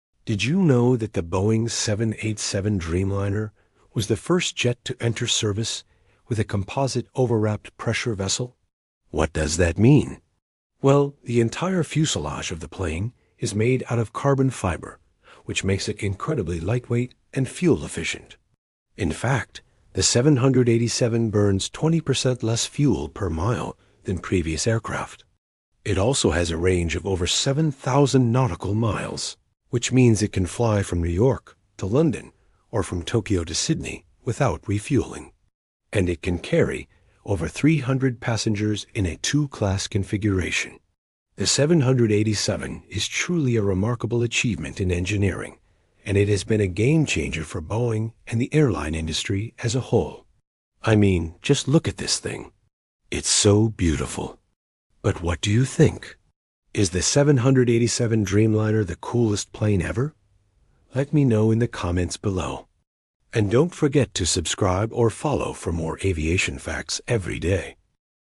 FLYING in a BOEING 787? HERE'S WHY IT'S BETTER! ✈️
Composite materials, super-efficient engines & insane range! ✨ Why Boeing's 787 Dreamliner was a game-changer for airlines & the planet. 🌍
Bigger windows, higher humidity, mood lighting & smoother ride! ✨ Experience why the Boeing 787 Dreamliner is a passenger favorite. ✈️ More comfortable long-haul!
Sleek wings, iconic raked wingtips & graceful presence! ✨ The stunning design & smooth power of the Boeing 787 Dreamliner. Pure aviation elegance! ✈️

 "Follow for more plane facts!" or "Like if you love the  Boeing 787!".